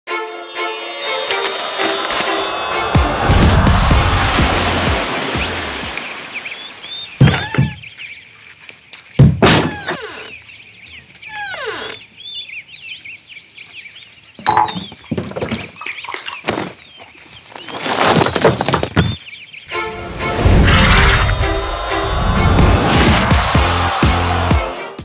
Little Foal - LEGO Friends - 41089 - Product Animation
Look after LEGO® Friends Little Foal with a stable, opening gates, fence, water trough with faucet, feeding bottle and a brush.